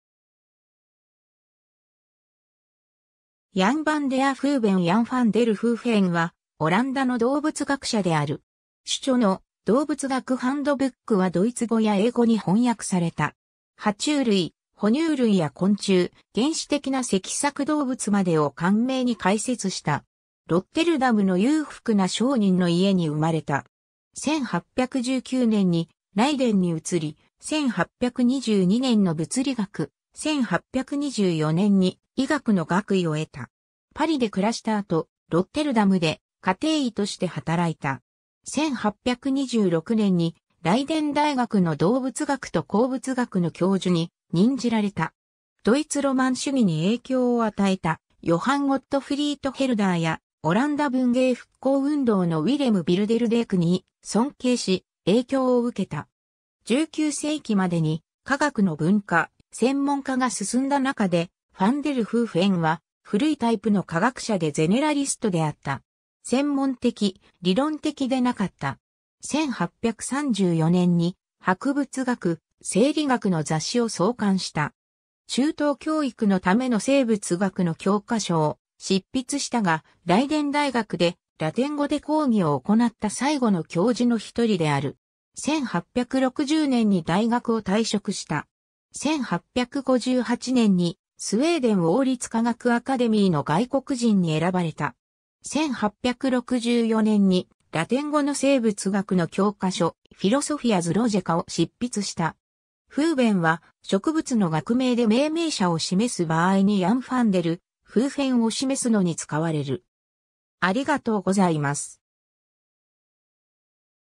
ヤン・ファン・デル・フーフェン
Jan van der Hoeven ヤン・ファン・デル・フーフェン（Jan van der Hoeven、1801年2月9日 - 1868年3月10日）は、オランダの動物学者である。
主著の『動物学ハンドブック』（"Handboek der Dierkunde" 1827–1833）はドイツ語や英語に翻訳された。
爬虫類、哺乳類や昆虫、原始的な脊索動物までを簡明に解説した。
 ロッテルダムの裕福な商人の家に生まれた。
1819年にライデンに移り、1822年の物理学、1824年に医学の学位を得た。
パリで暮らした後、ロッテルダムで家庭医として働いた。
1826年にライデン大学の動物学と鉱物学の教授に任じられた。
ドイツロマン主義に影響を与えたヨハン・ゴットフリート・ヘルダーやオランダ文芸復興運動のウィレム・ビルデルデイク（Willem Bilderdijk）に尊敬し、影響を受けた。
 19世紀までに、科学の分化、専門化が進んだなかで、ファン・デル・フーフェンは古いタイプの科学者でゼネラリストであった。
専門的、理論的でなかった。
1834年に博物学、生理学の雑誌を創刊した。
中等教育のための生物学の教科書を執筆したが、ライデン大学でラテン語で講義を行った最後の教授の一人である。
1860年に大学を退職した。
1858年にスウェーデン王立科学アカデミーの外国人に選ばれた。
1864年にラテン語の生物学の教科書、"Philosophia Zoologica"を執筆した 。
、 Hoevenは、植物の学名で命名者を示す場合にヤン・ファン・デル・フーフェンを示すのに使われる。